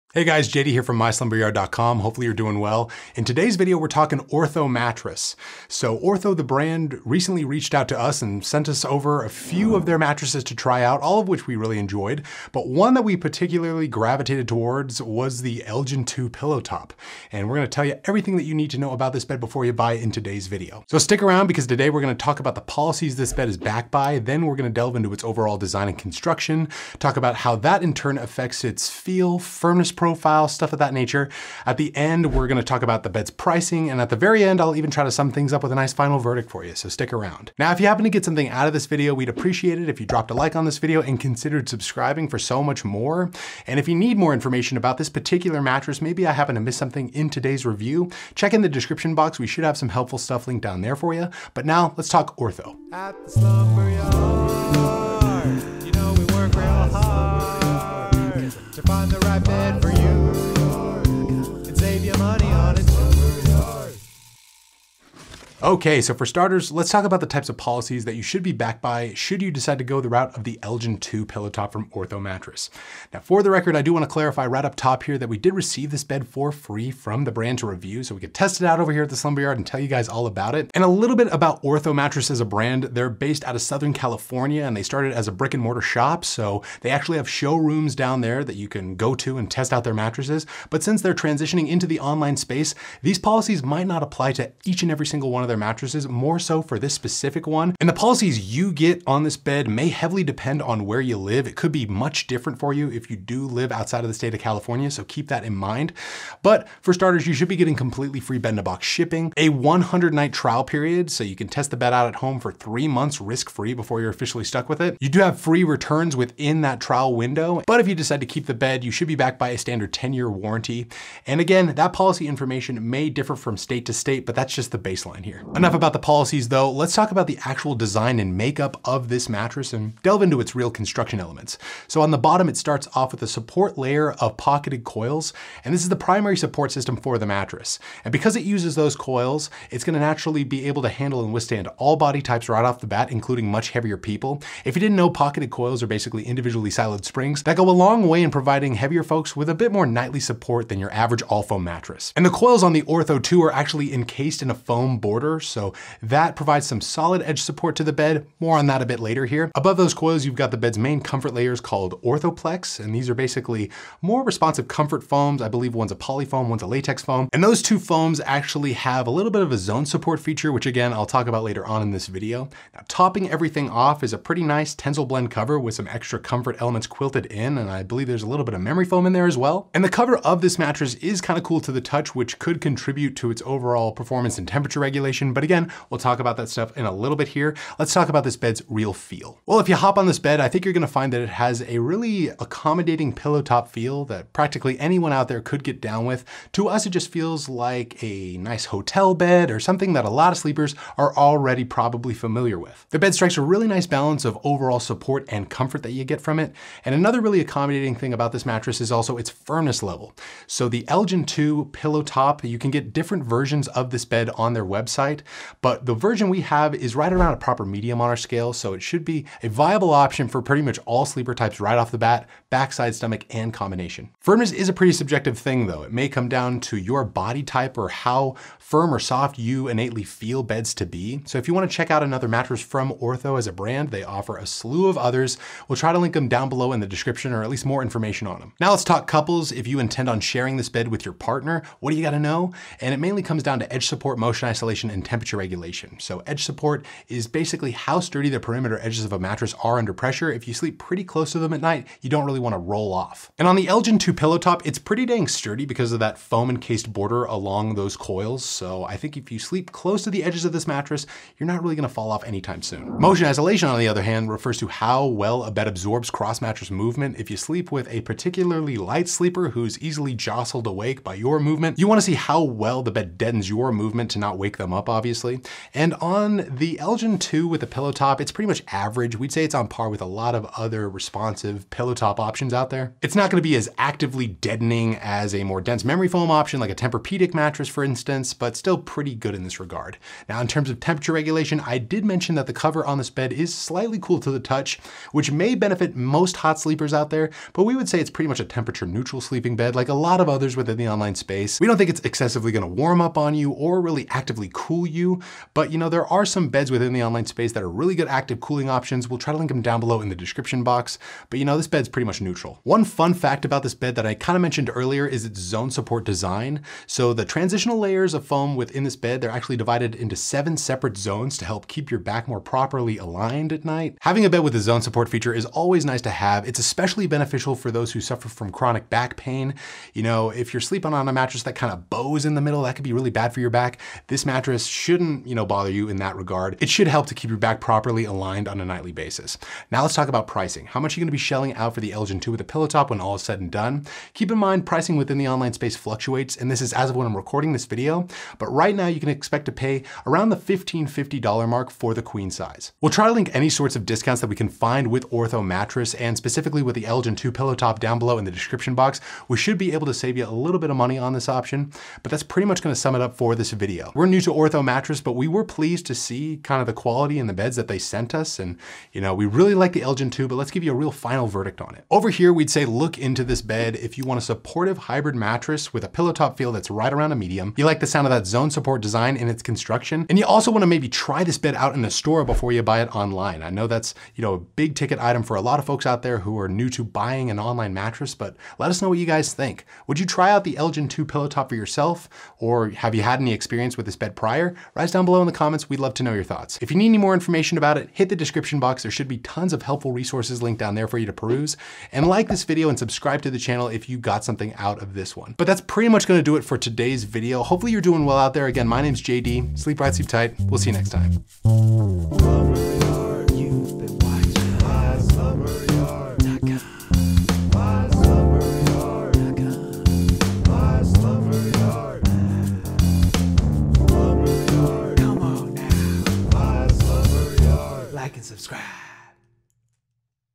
Ortho Mattress Review | Best Pillow Top Bed? (MUST WATCH)
Click "more" For Advertiser Disclosure ↓↓
     (Check Offers w/Link Above)

Ortho Mattress Review | Best Pillow Top Bed? (MUST WATCH). In this video, JD who is a Certified Sleep Science Coach, discusses our new review of the Elgin. This is a hybrid pillow top mattress made by a bed in a box brand called Ortho Mattress. Ortho Mattress also has other bed options on its website but in this video, JD reviews the Elgin Firm Pillow Top Mattress. JD touches on the mattress policies offered with this bed and also the construction of the mattress, how it feels and its firmness, and what sleeper type would be best for this bed (back sleepers, side sleepers, stomach sleepers, and combination sleepers). JD also discusses which body types (petite, medium and heavy) would enjoy this mattress. Overall, the Elgin Firm Pillow Top is a hybrid pillow top mattress that is a supportive and accommodating bed. Hopefully, it helps you choose your next online mattress purchase!

fzgE

▬ VIDEO CHAPTERS ▬
0:00 - Introduction
1:06 - General Mattress Policies
2:06 - Construction
3:07 - Feel
3:27 - Firmness
3:59 - Info for Couples
5:13 - Fun Facts
5:40 - Pricing
6:10 - Final Verdict: Ortho Elgin II PT Review
6:30 - Conclusion